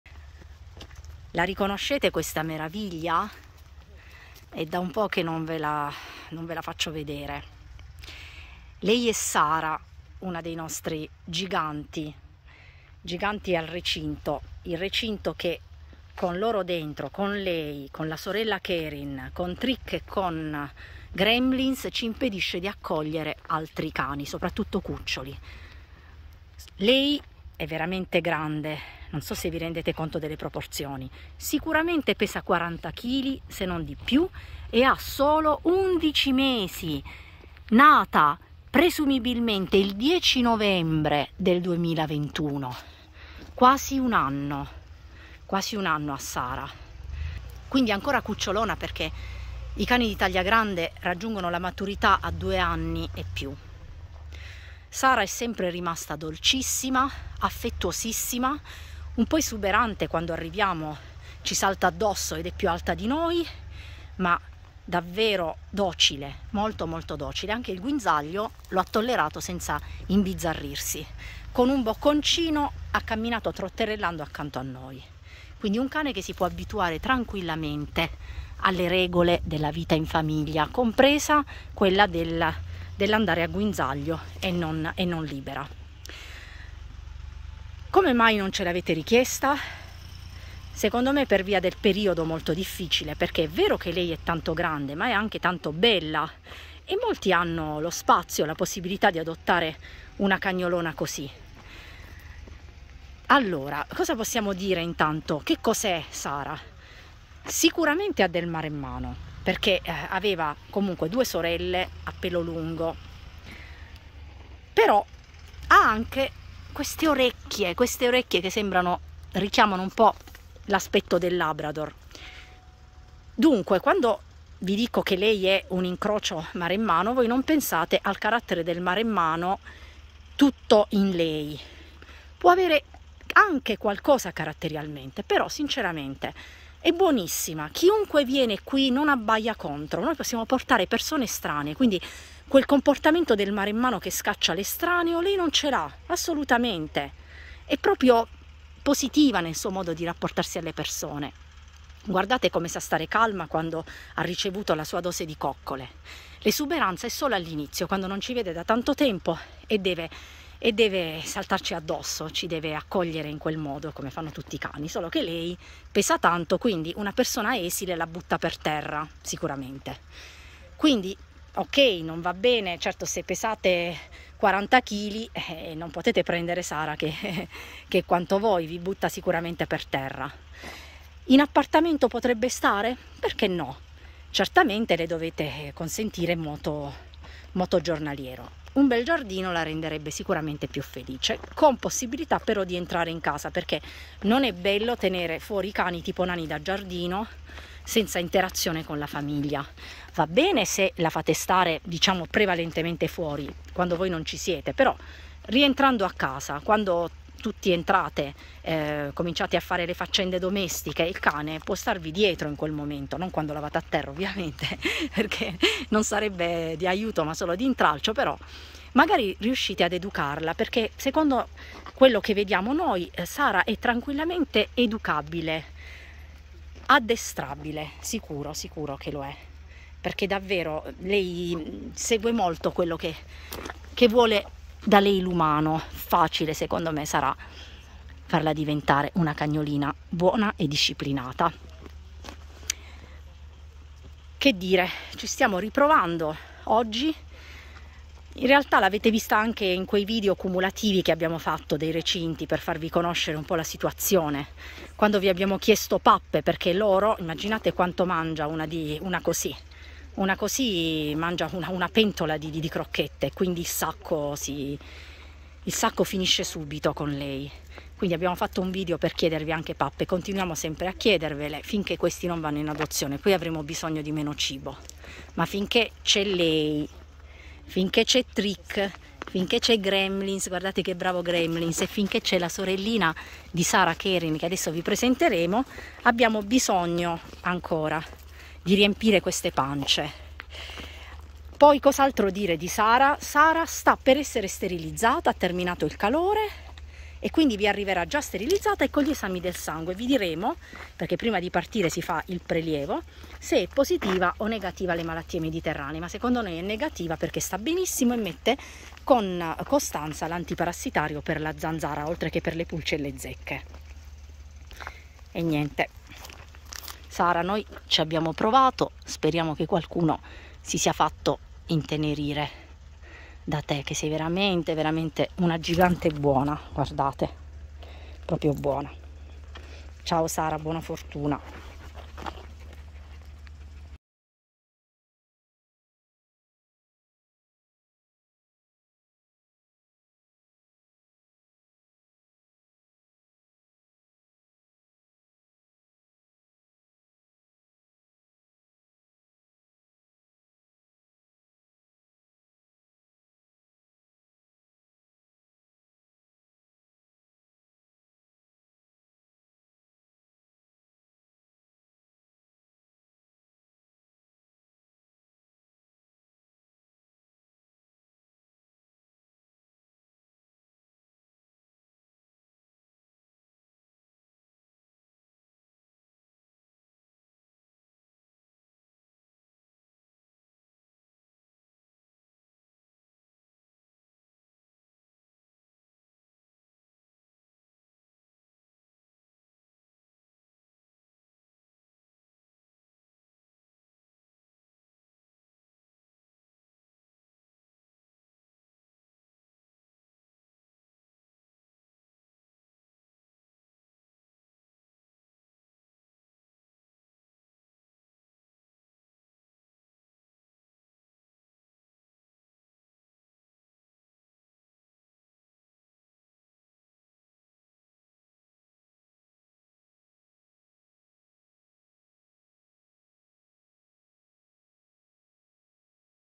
SARA la gigante dagli occhi a mandorla
Possibile che questa cagnolona sia dovuta arrivare a 11 mesi senza una richiesta di adozione, pur avendola proposta dall'età di 2 mesi? Eppure è andata così.
Lei ama più di ogni altra cosa abbracciare le persone e va d'accordissimo anche con i suoi simili, sia maschi che femmine.
Se le offrite un bel giardino dove poter sgranchire le sue zampone, ne sarebbe felice. Ma siccome non può fare a meno degli abbracci, sarebbe ancora più felice di stare in casa con voi e poi di uscire a passeggiare, correre e giocare.
Se siete alla ricerca di una femmina, giovane, di taglia decisamente grande e dal buon carattere, l'avete trovata, è Sara.